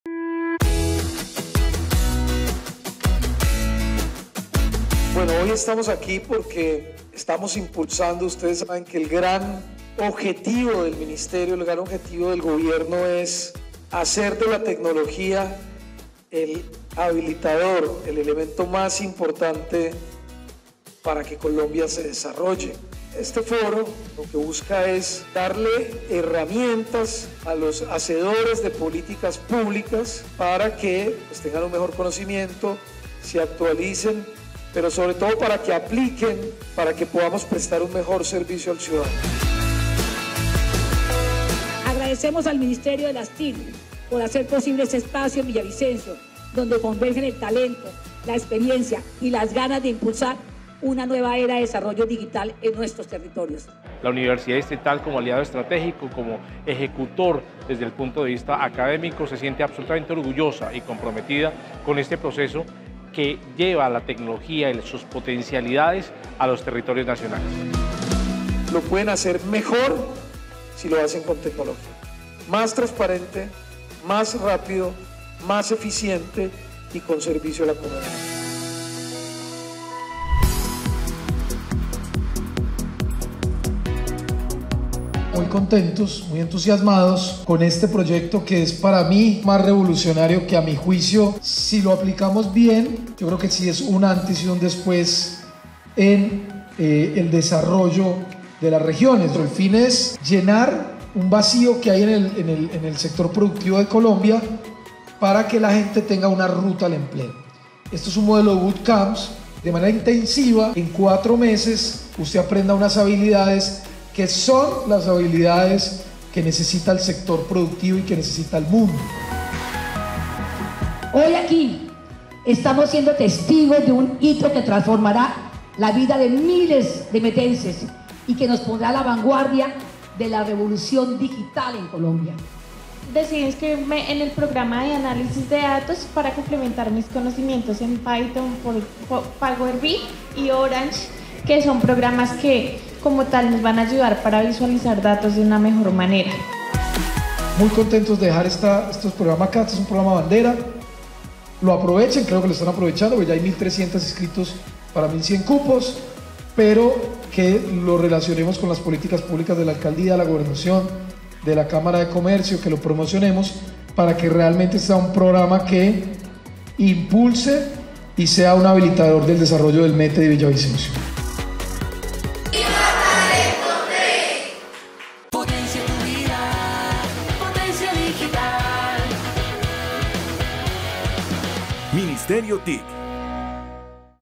Jornada llena de espacios de aprendizaje tecnológico con el Ministerio TIC
El ministro TIC, Mauricio Lizcano, llegó a Villavicencio para presentar diversas estrategias pedagógicas, encaminadas a generar un uso responsable de las TIC, formarse en habilidades digitales para la productividad, y avanzar hacia un gobierno departamental más incluyente, accesible e interoperable.

El lanzamiento de los Bootcamps 'Potenciando Territorios con la Innovación Pública', programa dirigido a gobernadores, alcaldes y secretarios TIC de todo el país, que incluye paneles, conferencias con expertos nacionales en tendencias y estrategias de gobierno digital, y talleres prácticos. Este curso hace parte del diplomado en Alta Gerencia TIC, diseñado para incrementar las habilidades digitales de los líderes de las entidades territoriales. Este espacio de Villavicencio corresponde a la región Andina, que abarca Meta, Cundinamarca, Boyacá, Casanare, Arauca, Guaviare y Vaupés.

Y el lanzamiento de Talento Tech. Aquí, más de 600 personas conocieron este programa de formación gratuita, que beneficiará a 1.124 habitantes del departamento, fortaleciendo sus habilidades digitales a través de una metodología intensiva de campos de entrenamiento intensivo en áreas como análisis de datos, programación, blockchain, arquitectura en la nube, inteligencia artificial y ciberseguridad.